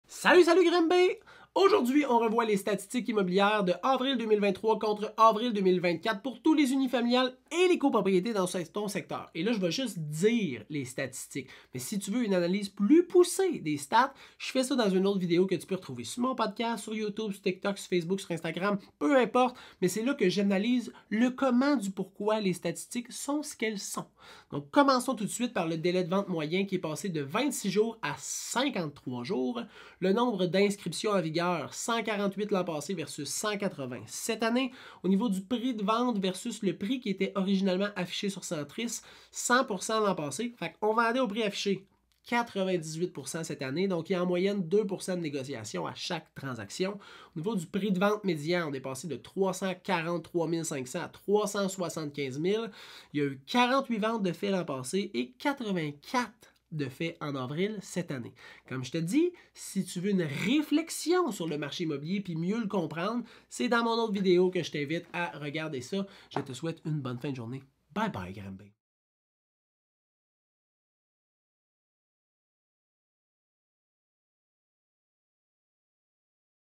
STATISTIQUES IMMOBILIÈRES pour le mois de avril 2024 à Granby 🚀 - Immobilier Estrie
Voici les statistiques immobilières pour Granby

Je vais analyser, pour toutes les unifamiliales et les copropriétés, cinq statistiques différentes  qui sont:

- prix de vente médian
- délais de vente médian
- inscription en vigueur
- nombre de ventes
- % de négociation moyen

Bonne écoute!